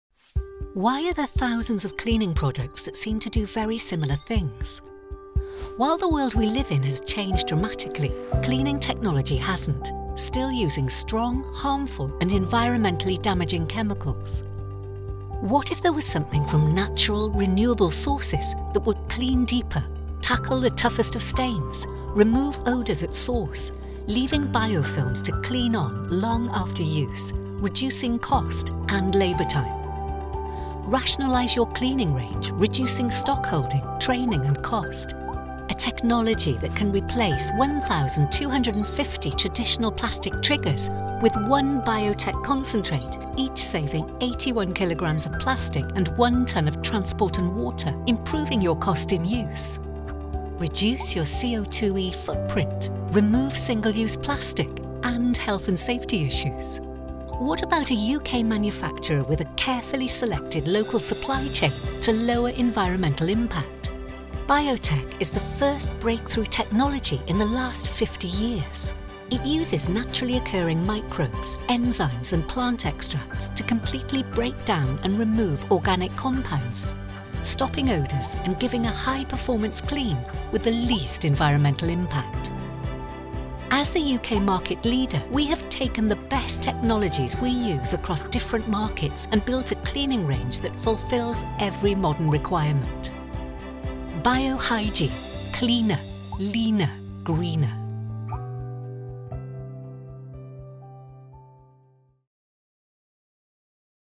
BioHygiene Introduction VIDEO
All Purpose Sanitiser utilises eco-friendly, naturally derived, mild surfactants and a natural fermentation extract to produce an environmentally-responsible and sustainable